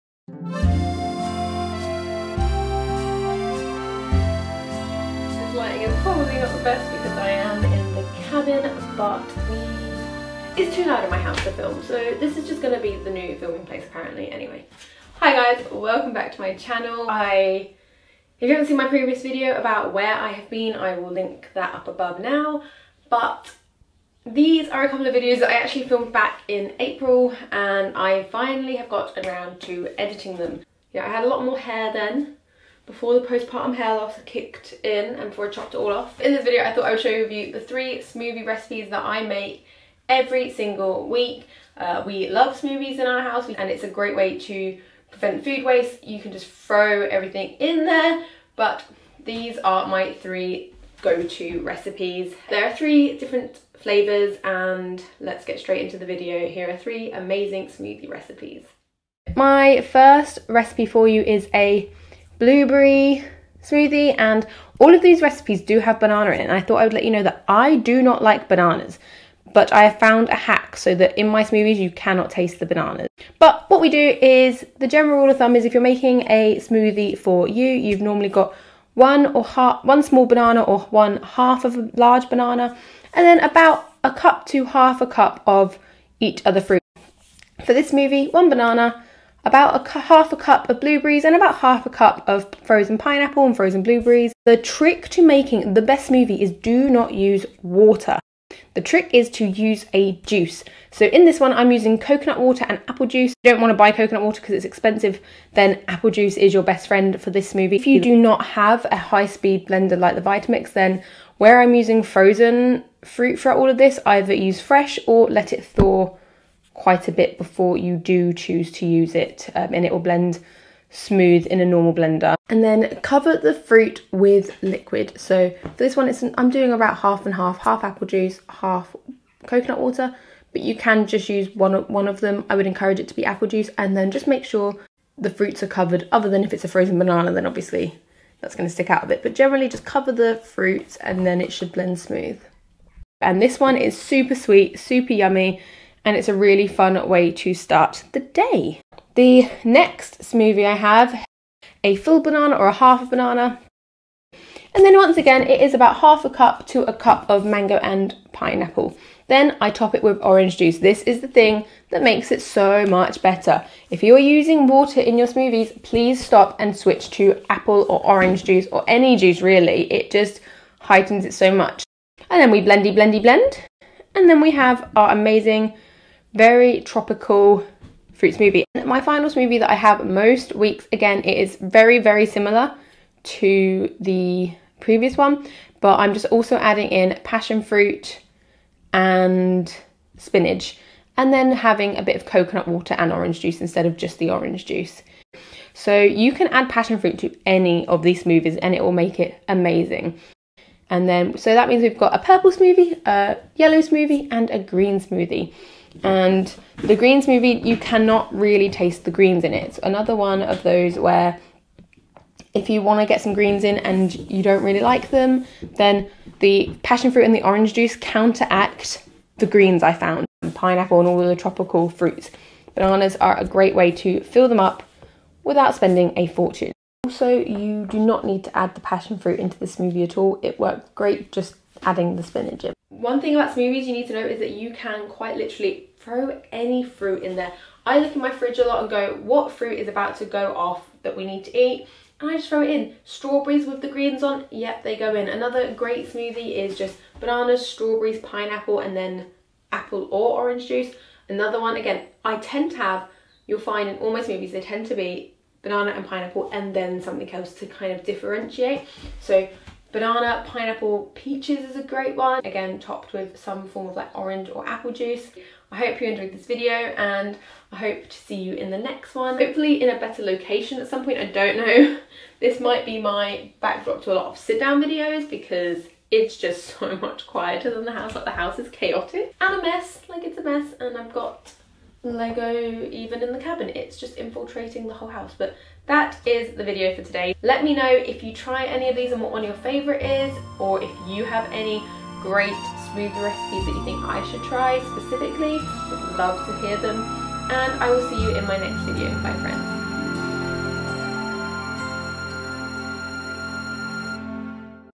The Best Smoothie recipes!!! | Kid Approved | Sensory processing Disorder Approved | Eps # 3
Firstly welcome and thank you for clicking on my face!

Use my Jam Doughnut link to start getting cash back!

This is part of a series I have decided to make over here on my channel of affordable, kid approved foods. That is also sensory processing disorder approved as one of my boys lives with SPD.